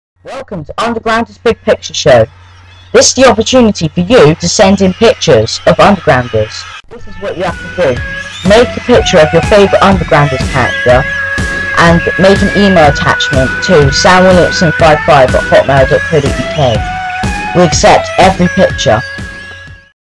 undergrounders big picture contest
all you need to do is make a picture about your favourite undergrounders character and send it in

also every picture that gets sent in goes on my show

if you need help put a comment